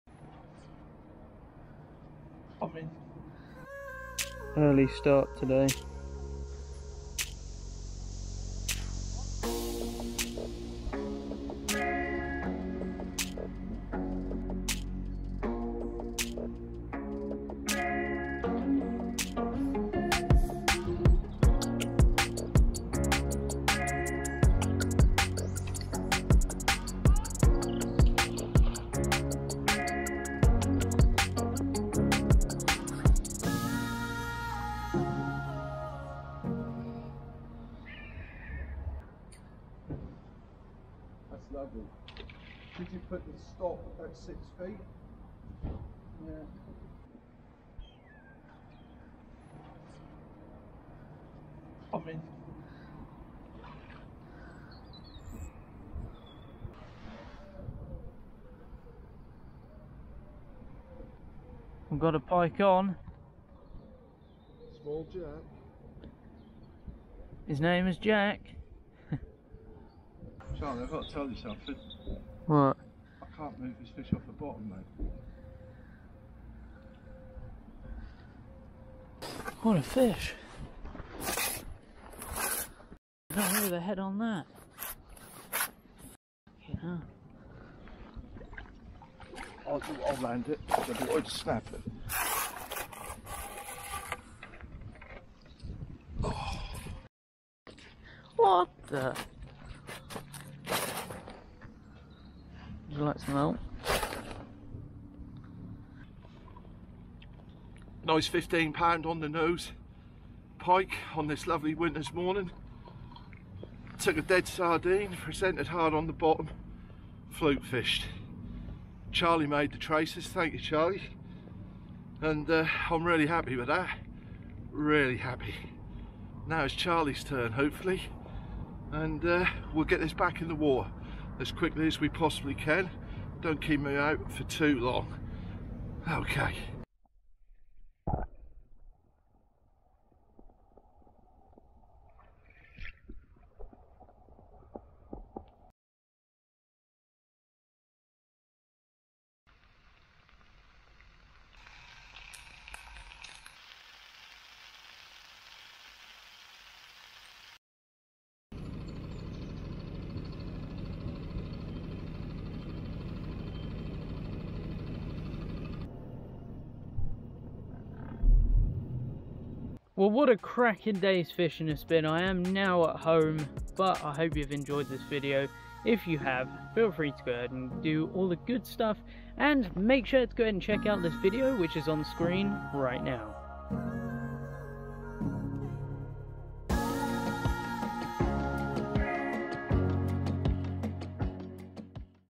BIG Pike caught on the broads! Cracking days fishing
This was a good days pike fishing! What a excellent looking fish.

This big girl was caught about 10 minutes after we had set up, unfortunately we had no more after that but what a success!

Also the pike was 15lbs